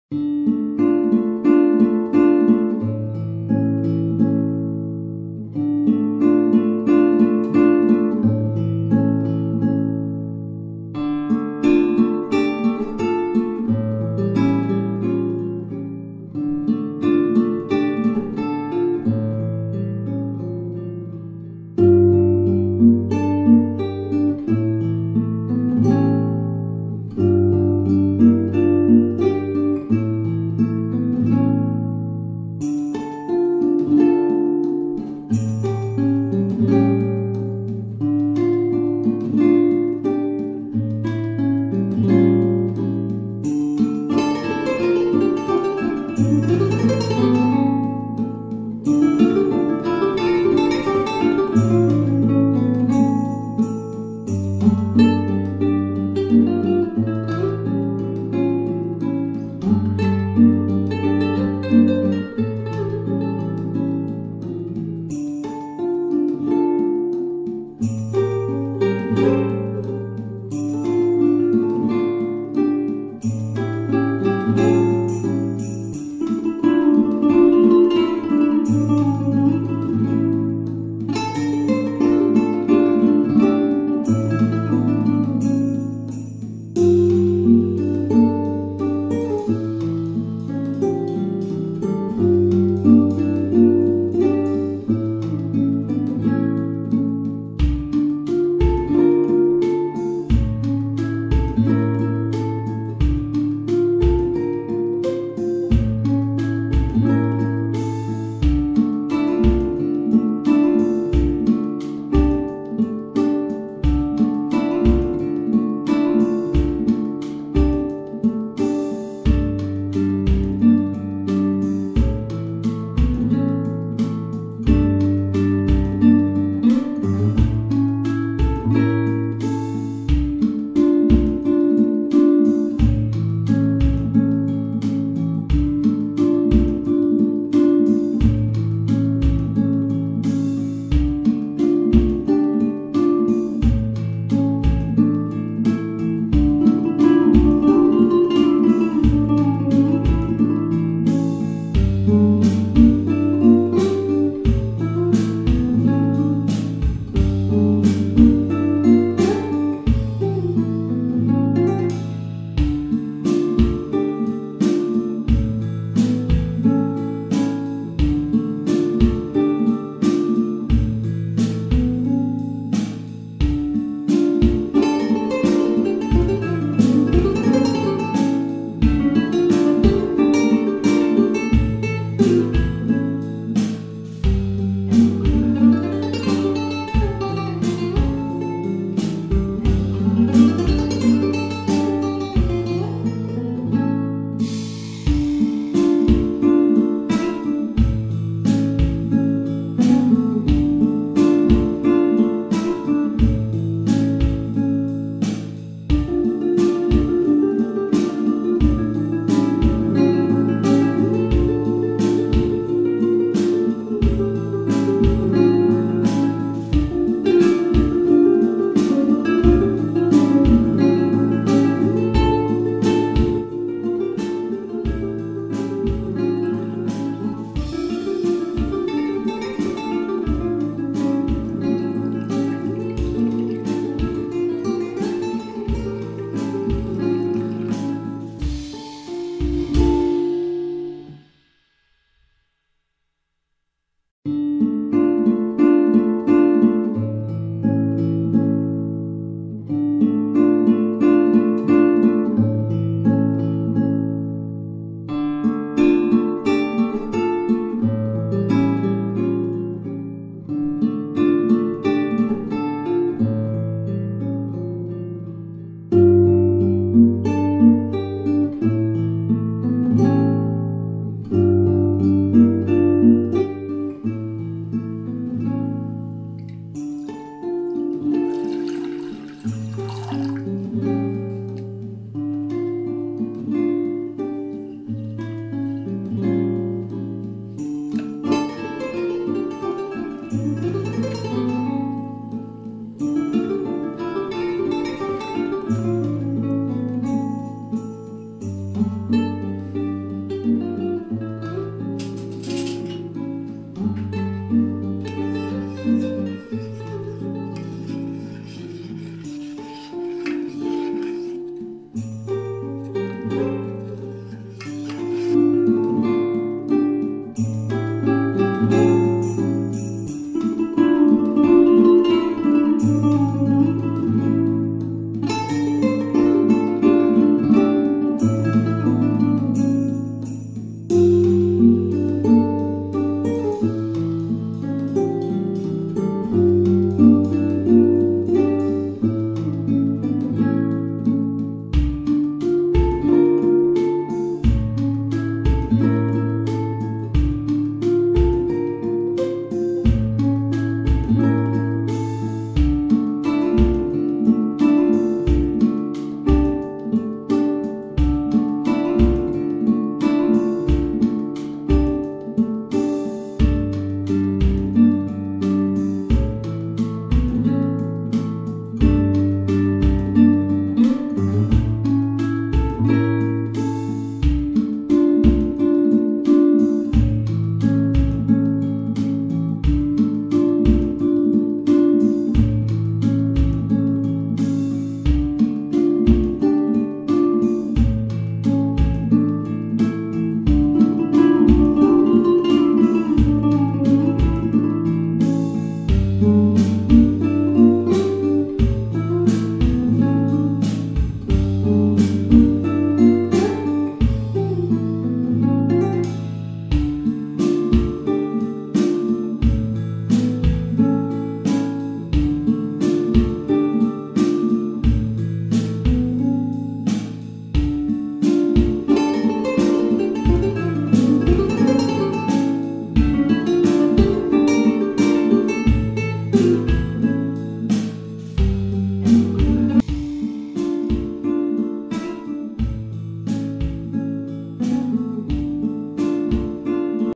Thạch Quýt
Ingredients:
6 large Tangerines
500ml of water
15gr Baking soda
ice water
250ml Water
60ml Lemon juice
60g Sugar
300ml Water
20g Gelatin
40g Sugar.

Công thức và cách làm mình tham khảo từ Yt của các bạn Hàn Quốc.